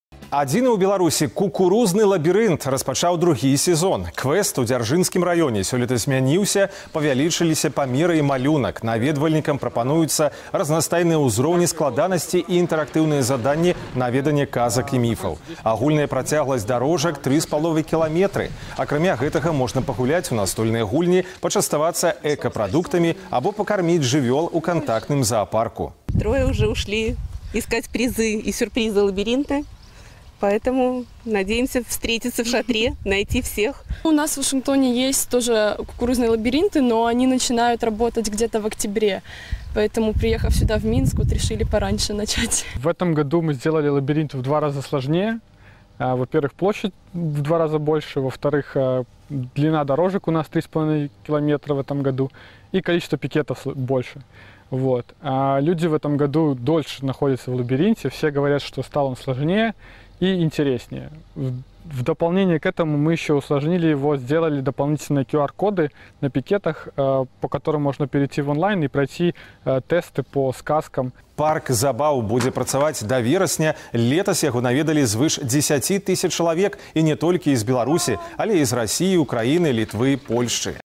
Адзіны ў Беларусі кукурузны лабірынт распачаў другі сезон. КРАІНА
Забаўны квест сёлета змяніўся - павялічыліся памеры і малюнак лабірынта, наведвальнікам прапануюць разнастайныя ўзроўні складанасці і цікавыя інтэрактыўныя заданні на веданне казак і міфаў.

Агульная працягласць дарожак - 3,5 кіламетра. Час для праходжання квеста не абмежаваны. Таксама ў парку забаў "Кукуполіс" можна пагуляць у настольныя гульні, пакаштаваць экапрадукты або пакарміць жывёл у кантактным заапарку.